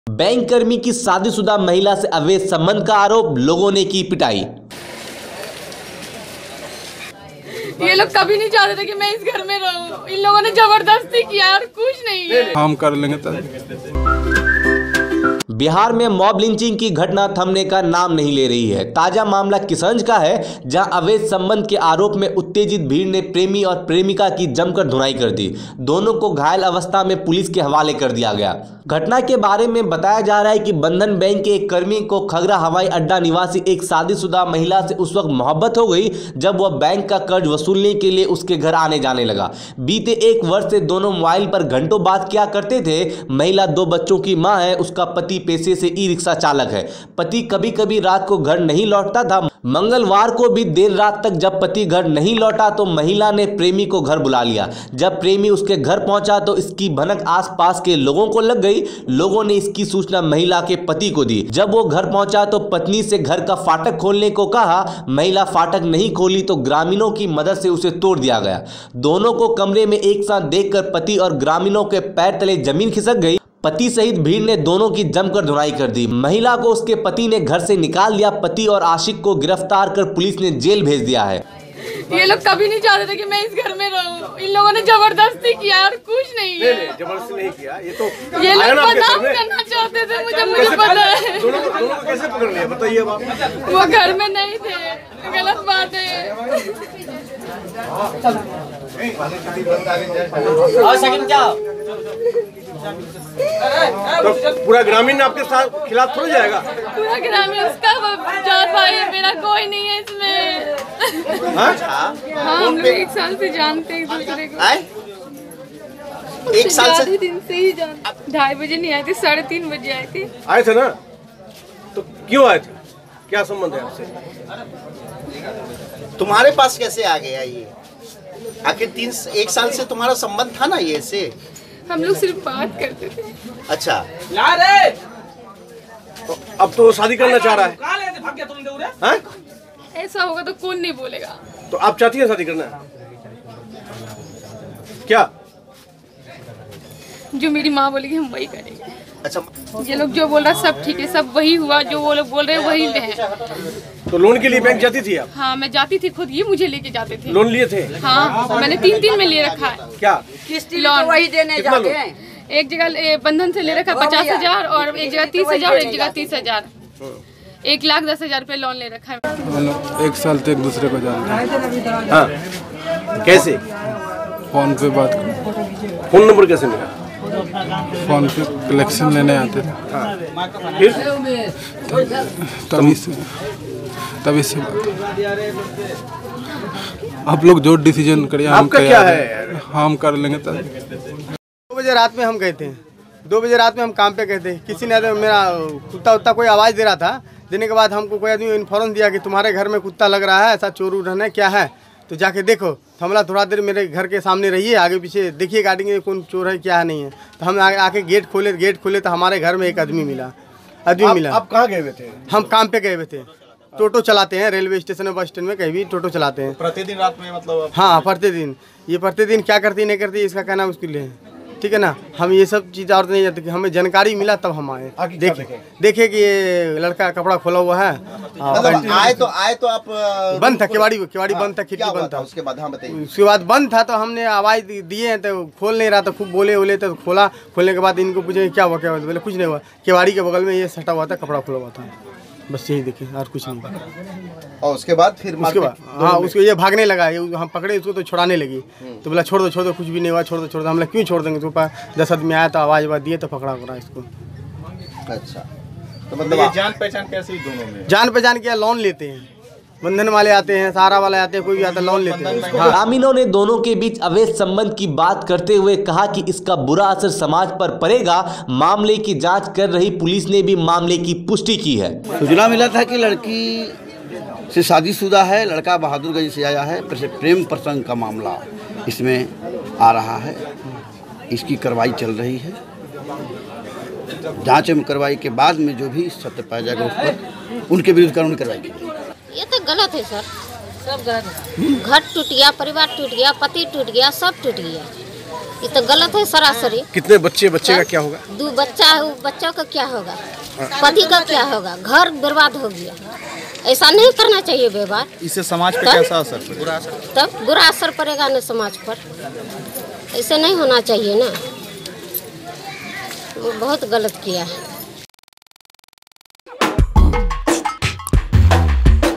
किशनगंज: शादीशुदा महिला के प्यार में पिट गया प्रेमी!
किशनगंज में प्रेमी-प्रेमिका के मॉब लिंचिंग का प्रयास, बैंककर्मी की शादीशुदा महिला से अवैध संबंध के आरोप पर लोगों ने की पिटाई। महिला के पति ने बैंककर्मी पर ब्लैकमेलिंग का आरोप लगाया है। पति और बैंककर्मी को पुलिस ने गिरफ्तार कर जेल भेज दिया है।